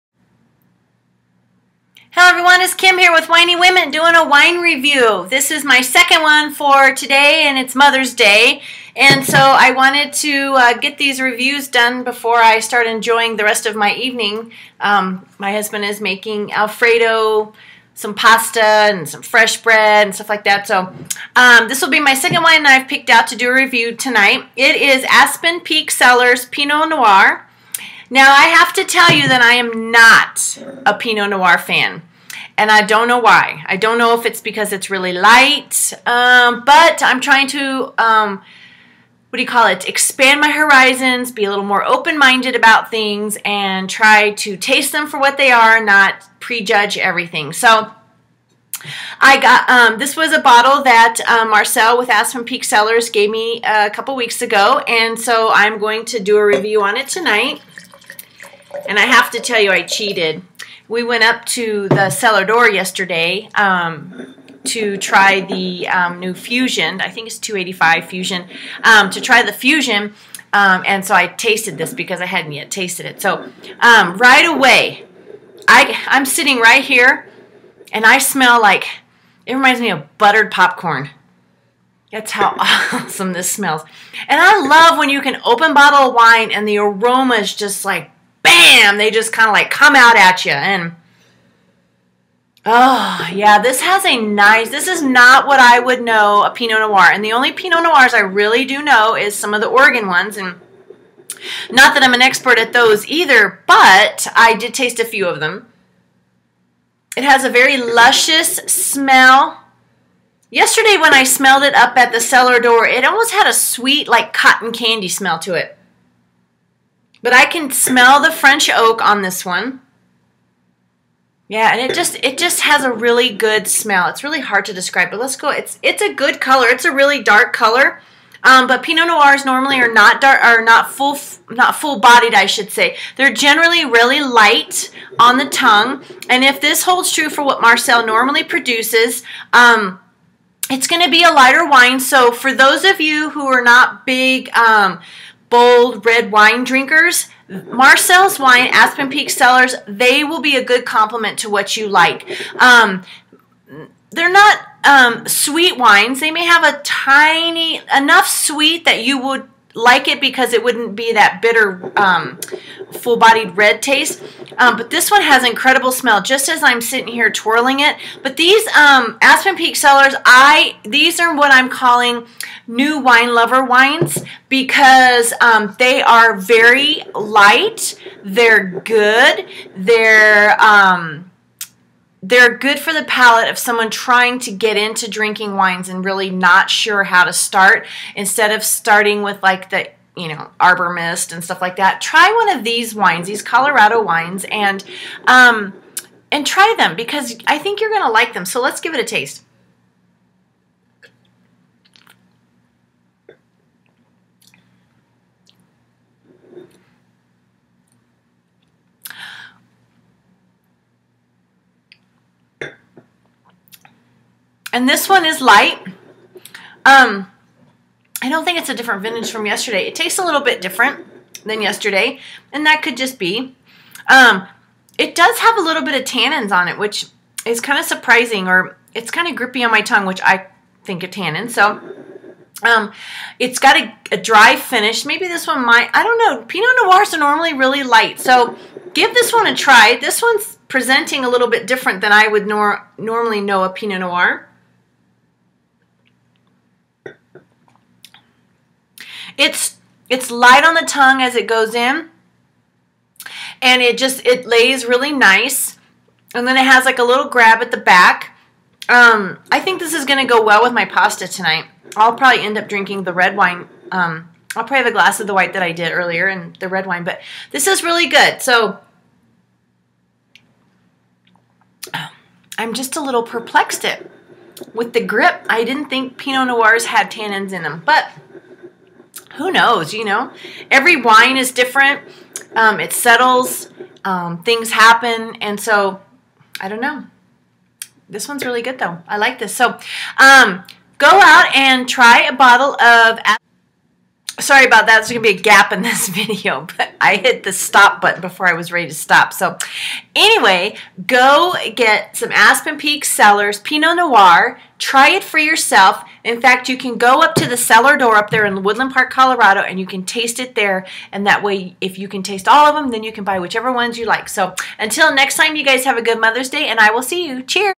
Aspen Peak Cellars Pinot Noir
Smells of French Oak, And Buttered Popcorn..Light on the tongue at the beginning and finishes with some slight tannins